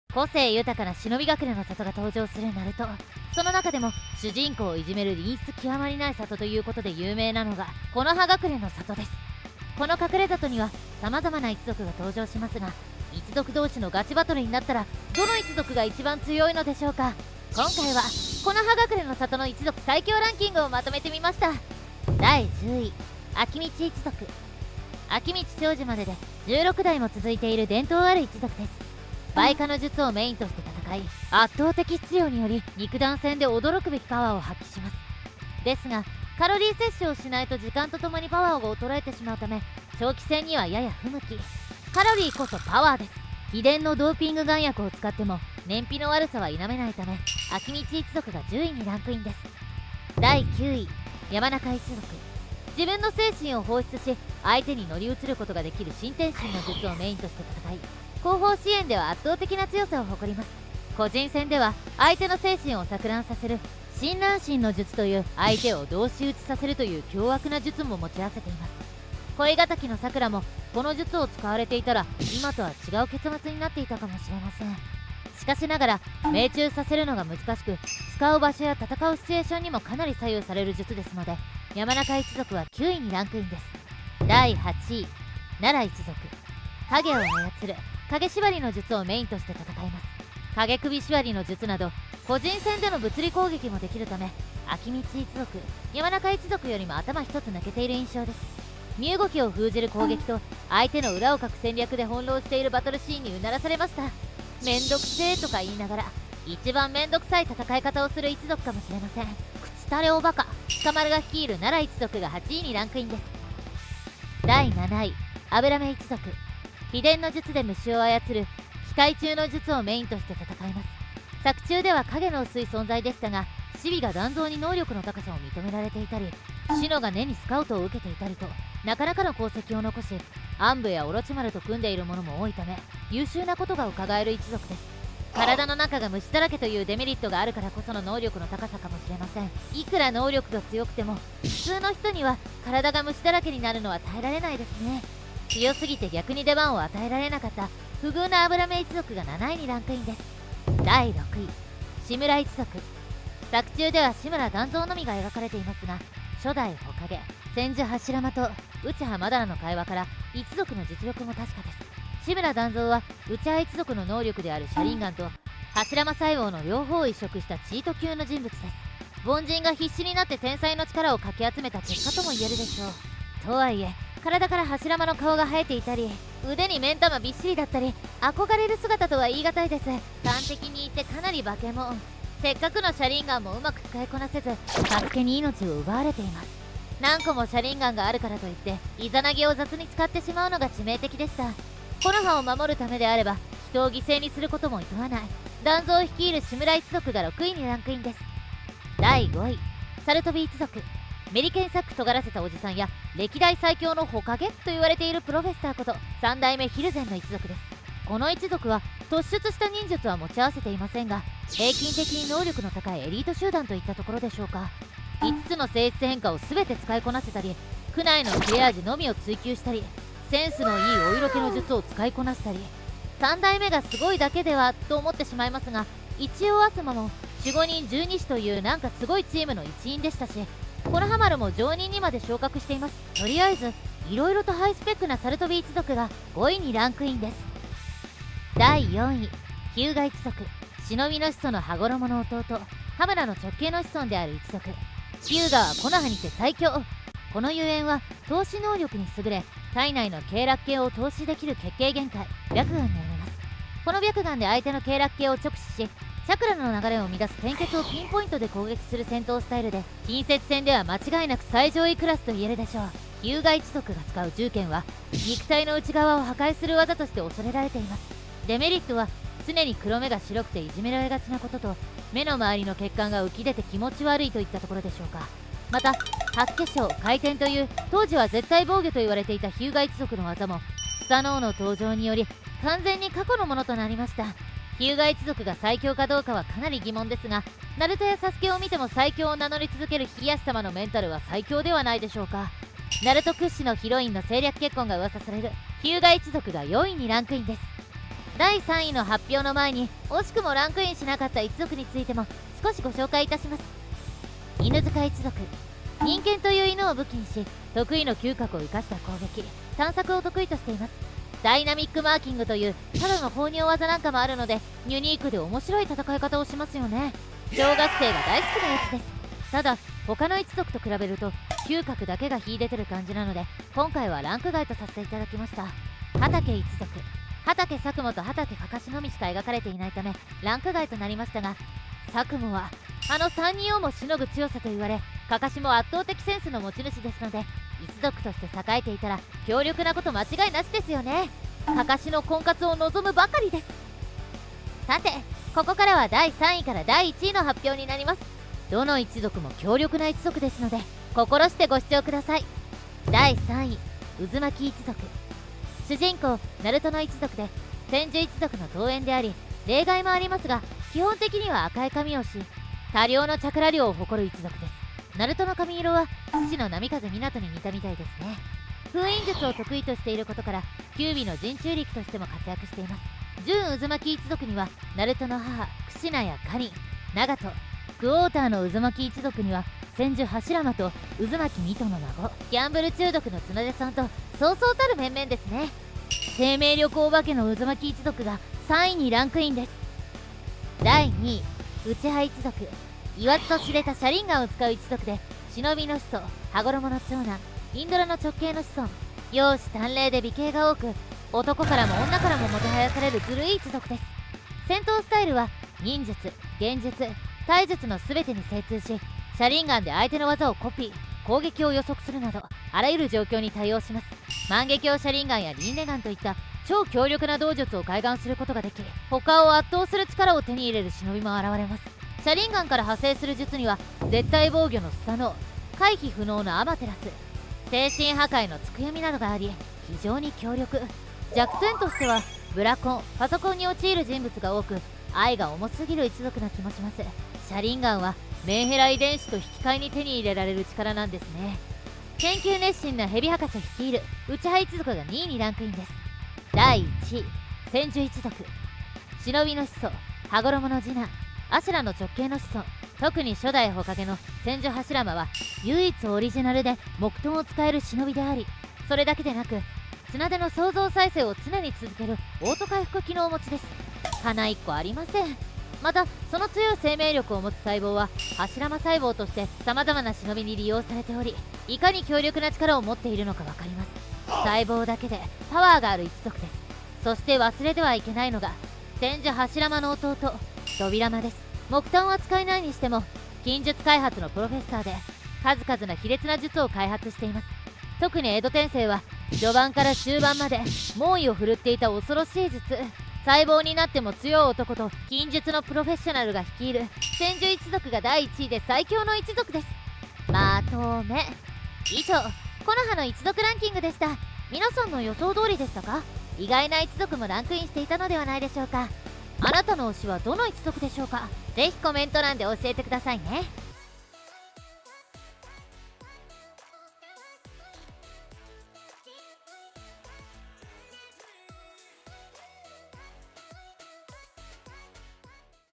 木ノ葉隠れ最強の一族決定だってばよ！【ナルト疾風伝】
先に言っておきます。日向一族ではないです。

・【木ノ葉の闇】うたたねコハルと水戸門ホムラの悪行まとめだってばよ